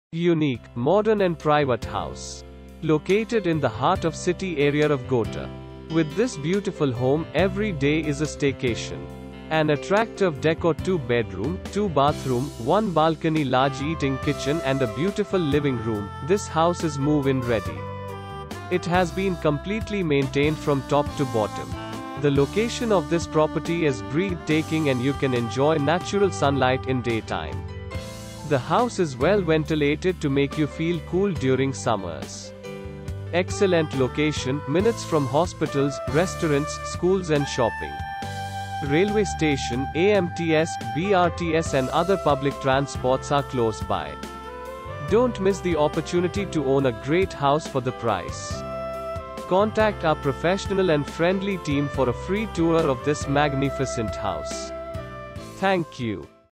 Royal Residency , Gota
Unique, modern and private house. Located in the heart of city area of Gota. With this beautiful home, every day is a staycation. An attractive decor two Bedroom, two bathroom, one Balcony large eating kitchen and a beautiful living room, this house is move-in ready! It has been completely maintained from top to bottom. The location of this property is breathe taking and you can enjoy natural sunlight in day time. The house is well ventilated to make you feel cool during summers. Excellent location, minutes from hospitals, restaurants, schools and shopping. Railway Station, AMTS, BRTS and other public transports are close by. Don't miss the opportunity to own a great house for the price! Contact our professional and friendly team for a free tour of this magnificent house! Thank You.